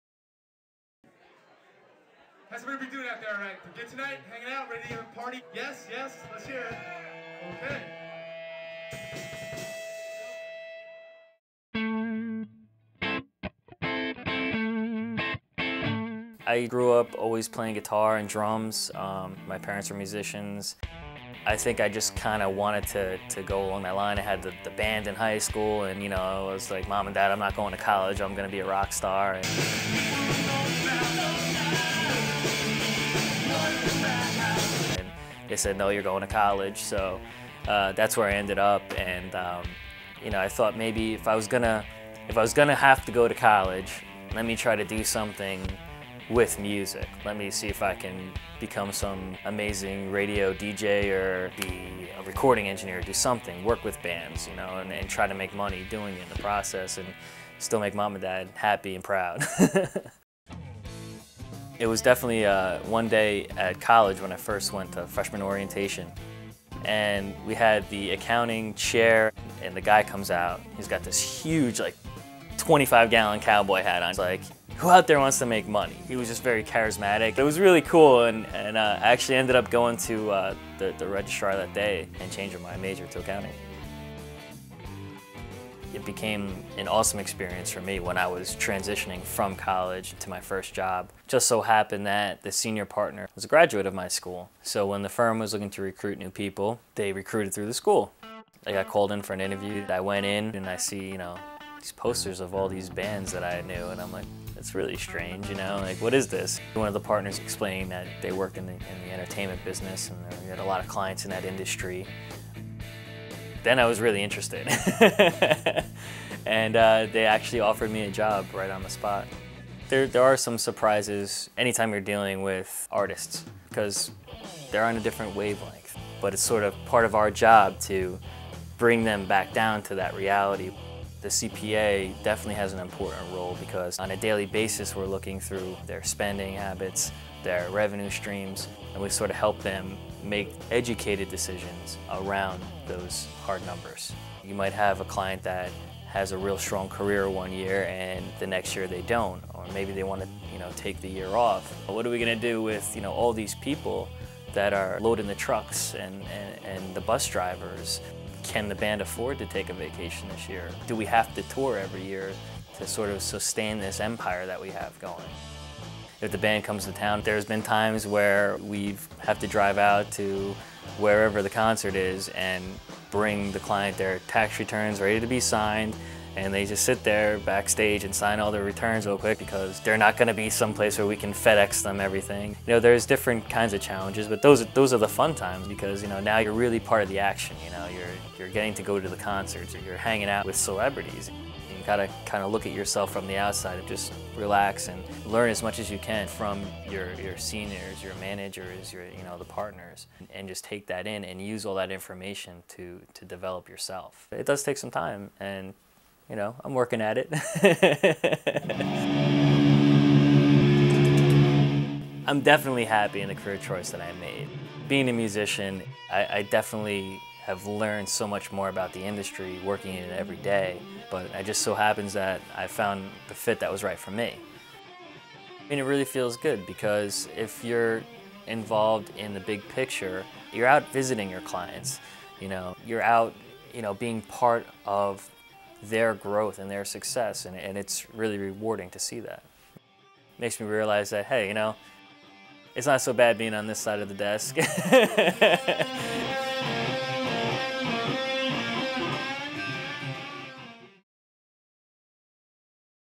AICPA - Profile: Ray Pinglora, III, CPA (Professional Profile) (2010)
Dave Maresca:
"CPAs and Rock n' Roll...not a common combination, right?  Wrong.  When a career in music seemed like a waning childhood dream, and the looming world of adulthood was waiting for him to graduate college, Ray Pinglora, Jr. was introduced to the world of accounting...and that an accounting alumnus from his school just happened to be the CPA for legendary rock band KISS.  Ray Pinglora, III, CPA is proof positive that a career as a CPA can still lead to working in any industry...even one you thought unattainable."

Producer - Project Coordination / Interviewing / Directing / Scripting / Editing: Dave Maresca

Dave Maresca was a Producer / Director with the AICPA Video team for 16 years, from Jan. 2004 - Jan. 2020.